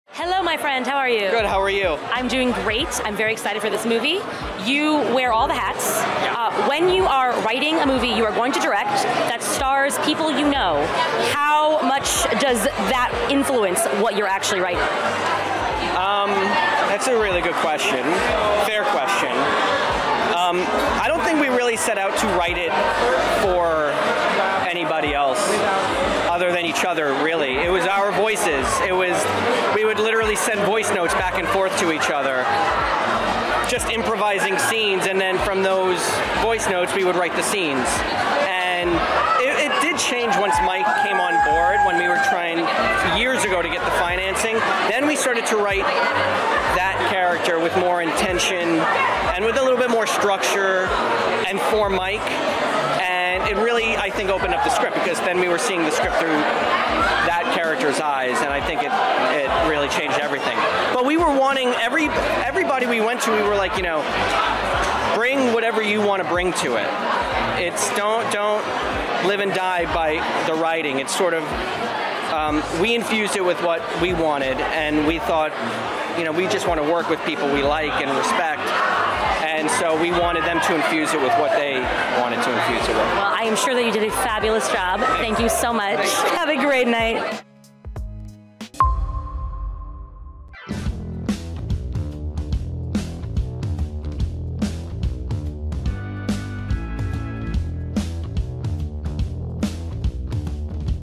Michael Angarano Talks Sacramento At Tribeca Film Festival
Sacramento premiered June 8 at Tribeca Film Festival and will release later this year through Vertical.